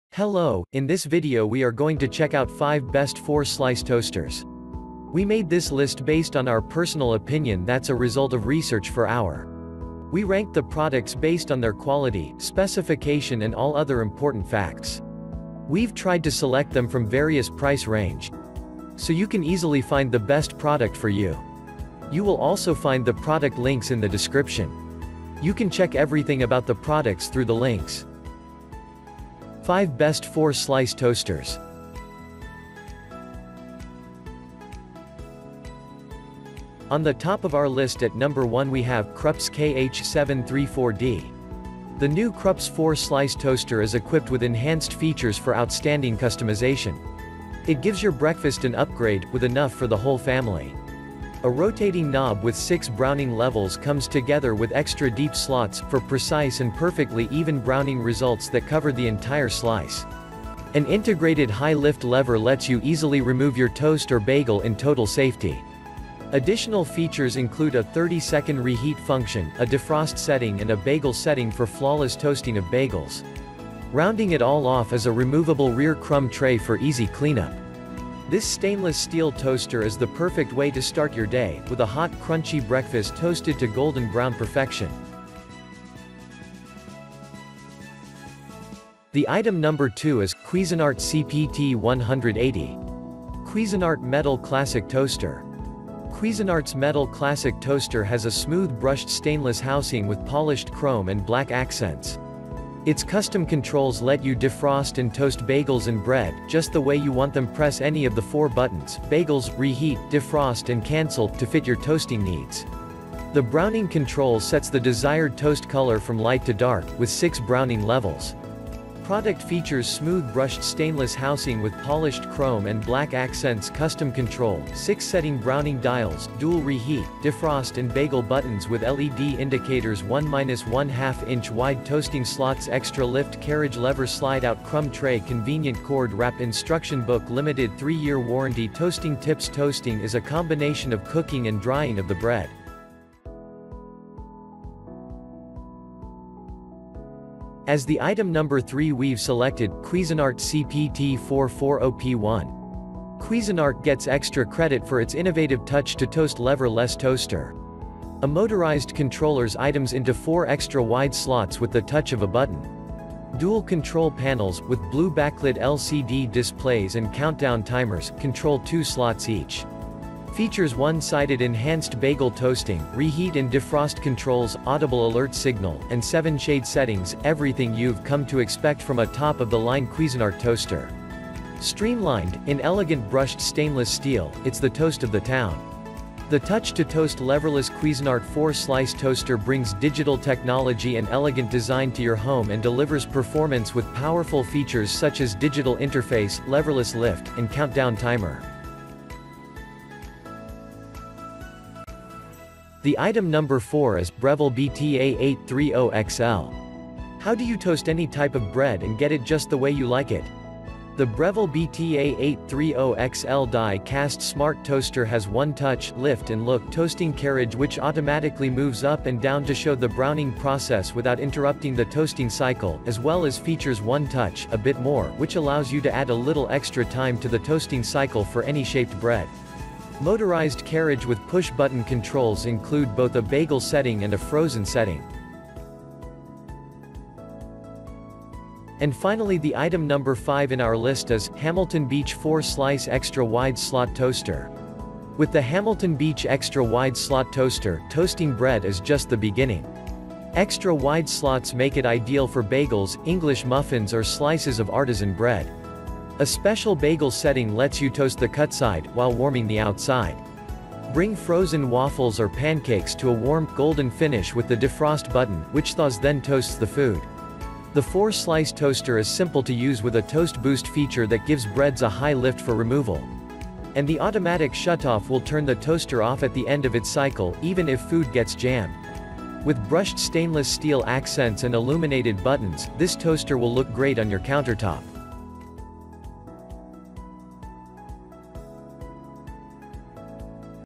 Best 4 Slice Toaster - Top 5 4 Slice Toasters
► Links to the 4 Slice Toaster we listed in this video:
►1.KRUPS KH734D
►2.Cuisinart CPT-180
►3.Cuisinart CPT-440P1
►4.Breville BTA830XL
►5.Hamilton Beach 4-Slice Extra-Wide Slot Toaster

In this video, we listed the 5 Best 4 Slice Toaster available in the market for this year. We made this list based on my personal opinion and i tried to list them based on their quality, durability, price,  and more.